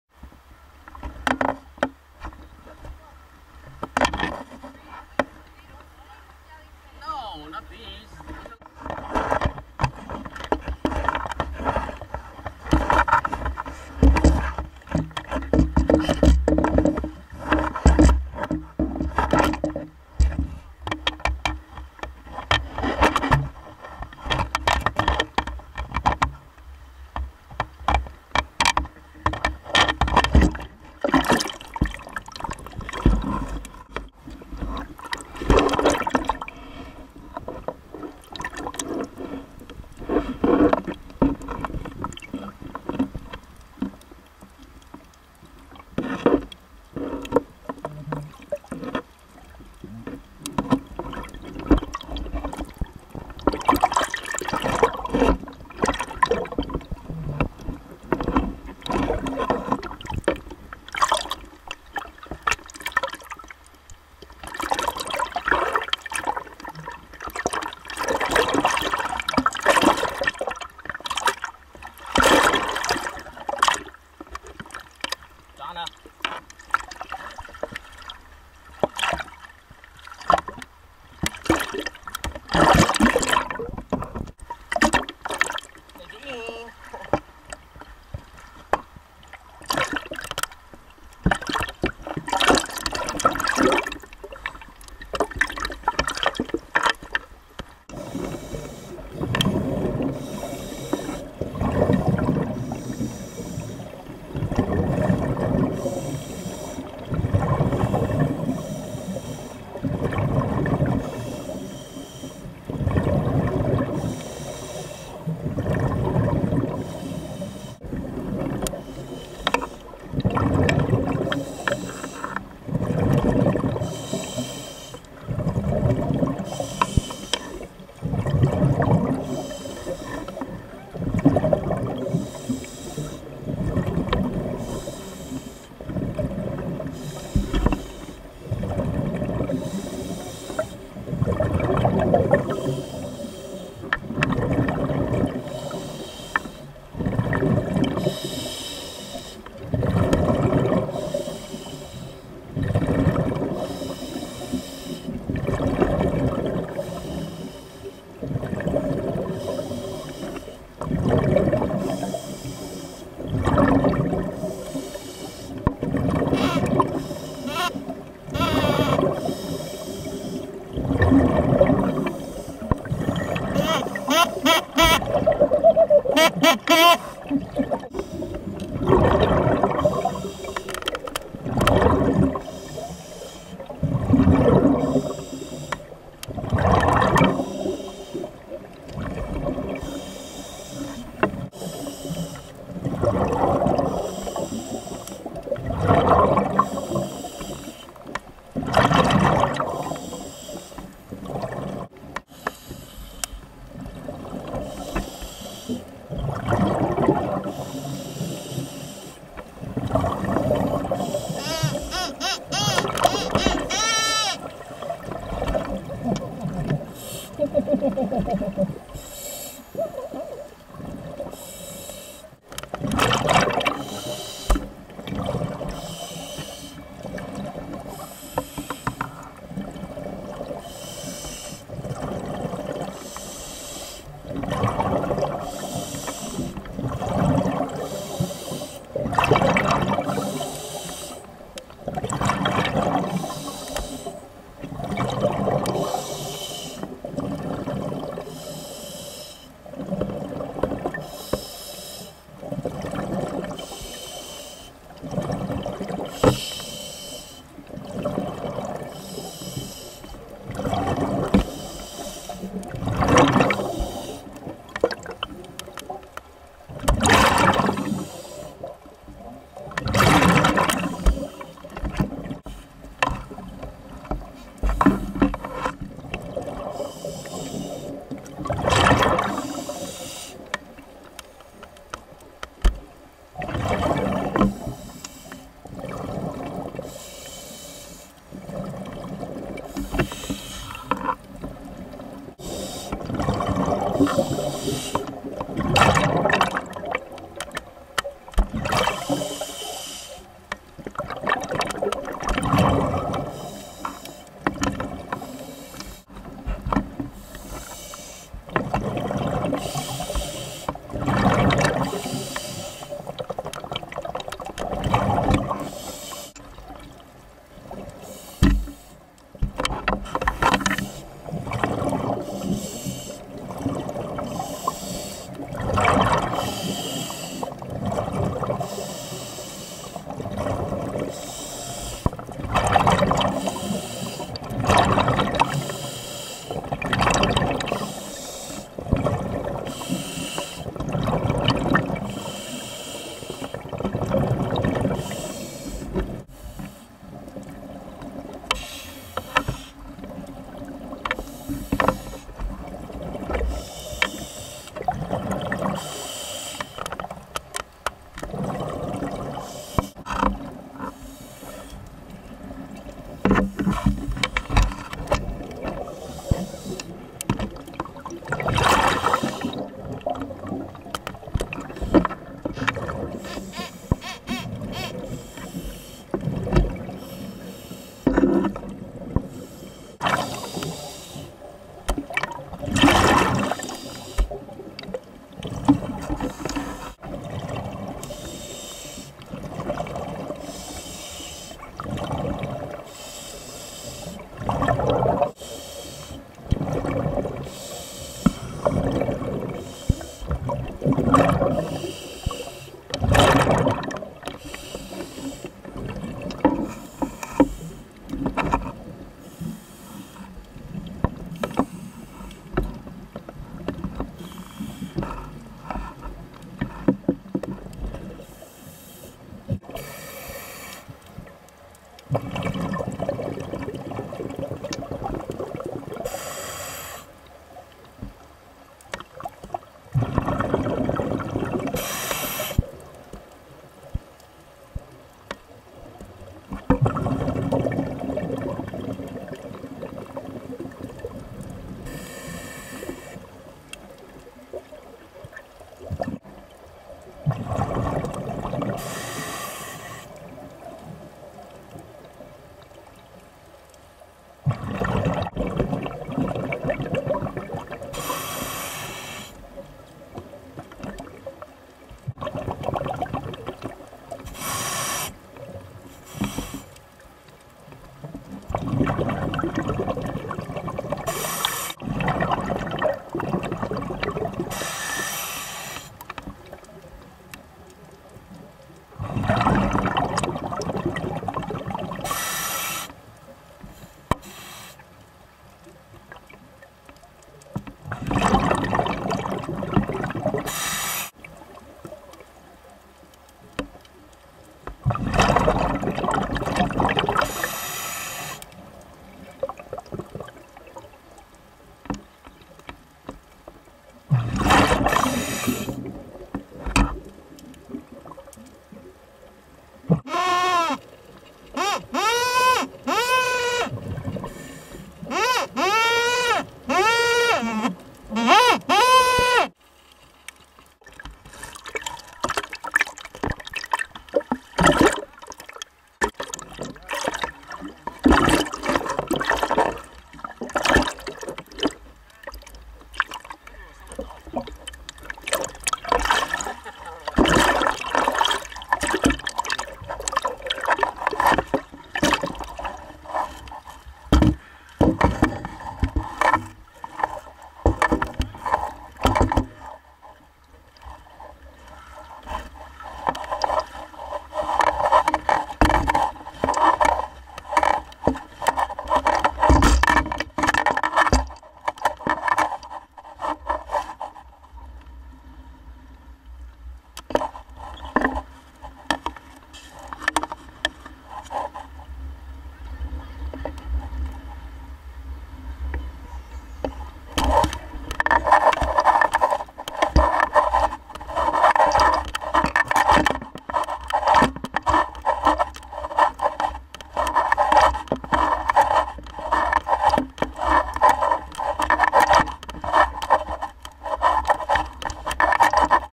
Nadine & Adnan Discovery Dive , Aqaba Jordan (View in HQ)
**Adnan & Nadine, hey glad you both decided to try Diving Out !...Ah S__  No Biggie. Next stop get certified eventually.. -Joe

Location: Aqaba Jordan, Diving at the Royal Jordanian Dive Club.
, friendly staff at hotel, but a lot much nicer diggs around. Has a very convenient pier to launch from for diving vs going out on a boat.

Camera Used: Sony HDR-SR12 / High Defintion
Camera Underwater Housing Unit from Iklite Digital Casing, Maximum operating depth is 200 feet (60 m)

BCD's used by divers shooting video Aqua Lung/SeaQuest Desc BCD ProQD with i3 .